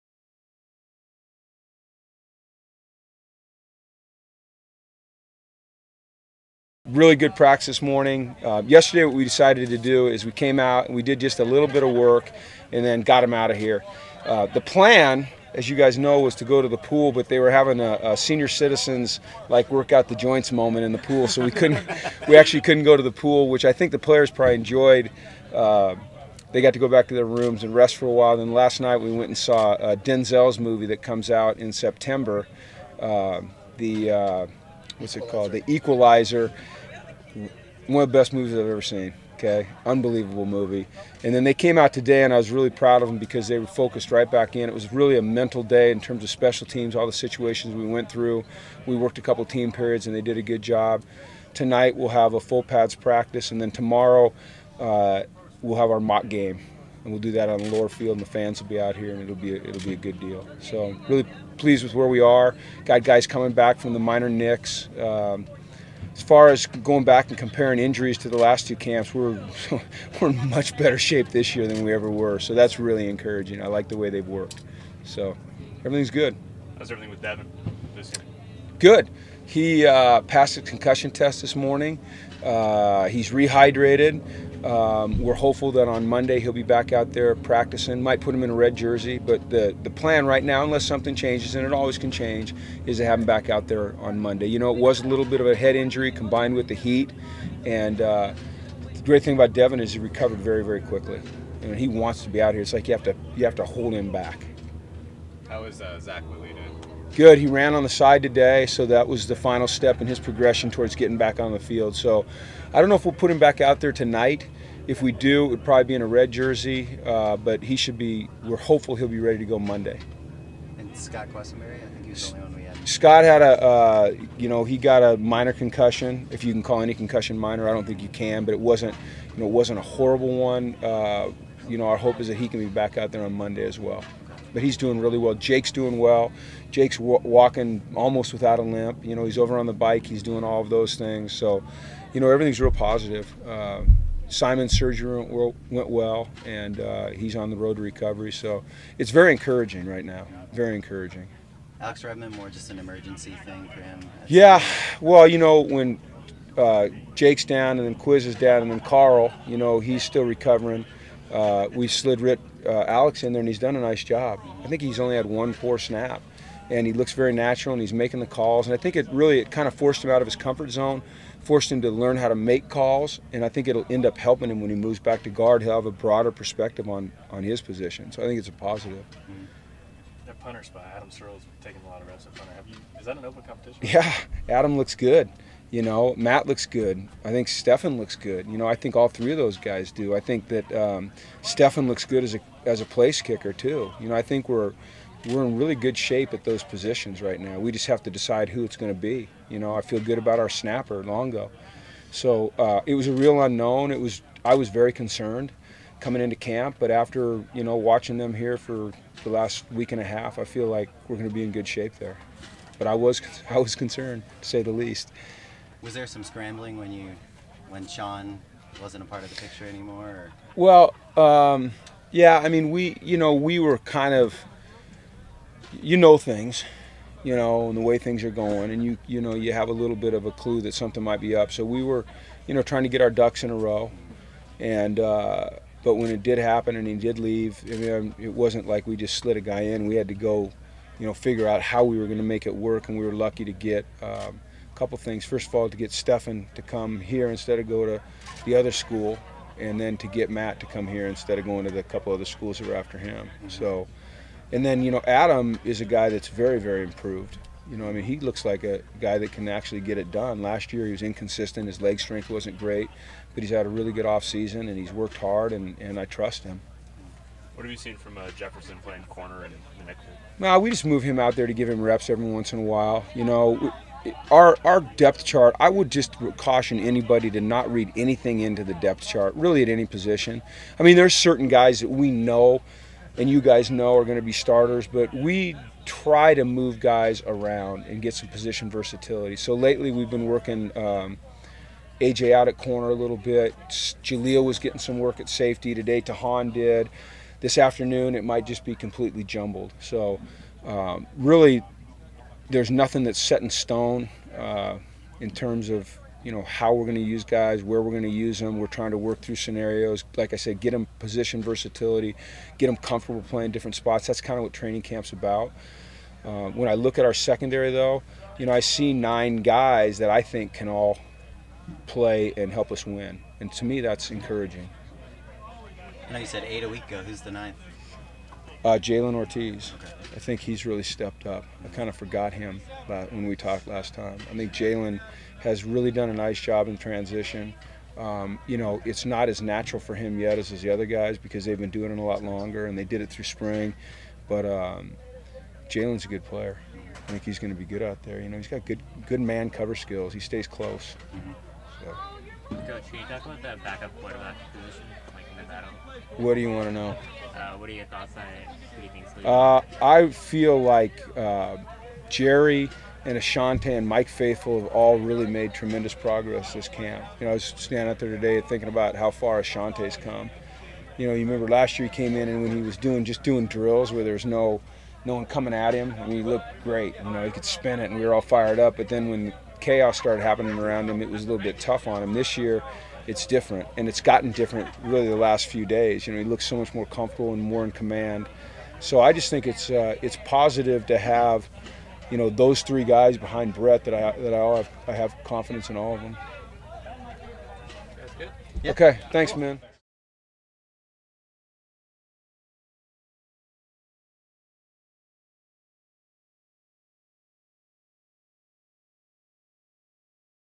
Jim Mora After Practice, 8-15-14
Coach Mora meets the media after Friday morning's practice in San Bernardino.